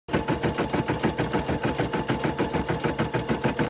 Embroidery machine - Donald Duck
This video shows how we achieve perfect embroidered results. We can embroider a range of Disney characters.
If there's a specific image/character you want embroidered we can do this!